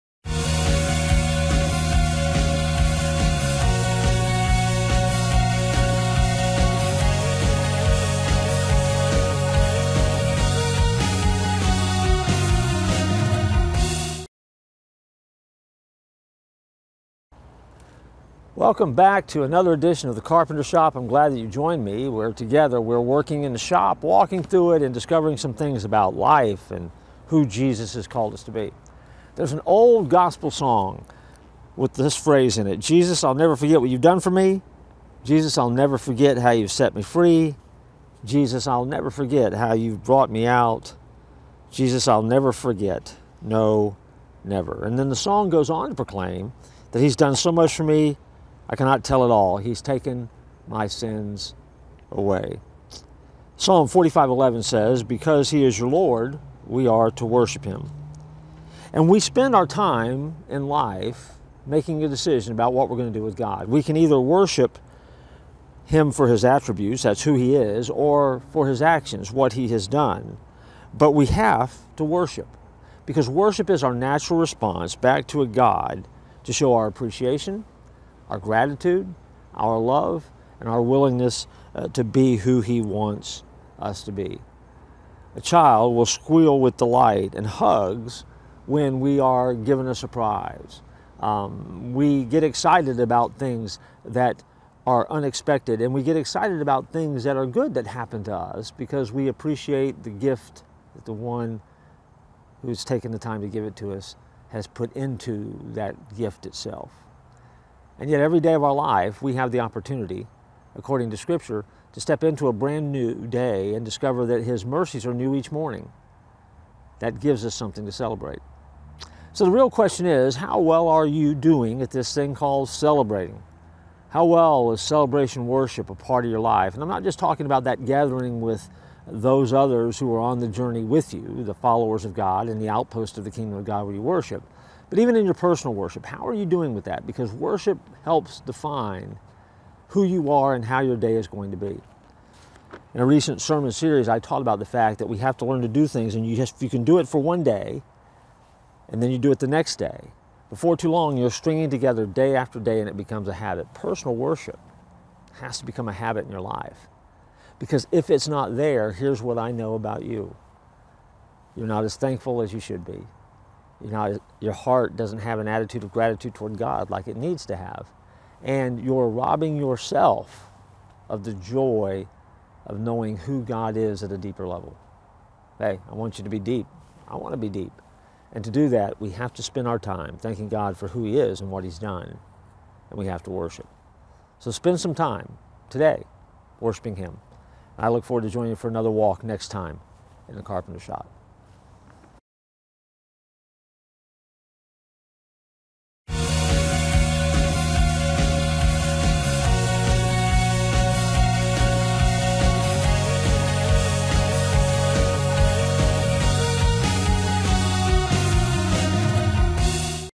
A Walk Through the Carpenter's Shop - S3 - Episode 24
In this episode, we take a look at how you are doing with worship and how thankful you are.